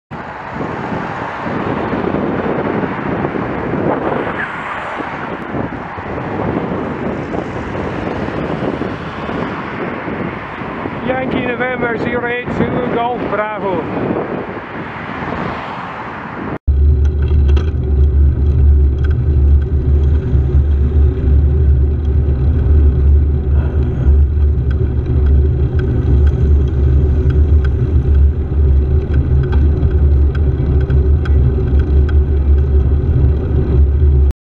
YN08ZGB if at first you don't succeed
Didn't manage a head on crash first time, tries again.

Thursday 30th November 2017 approx 17:45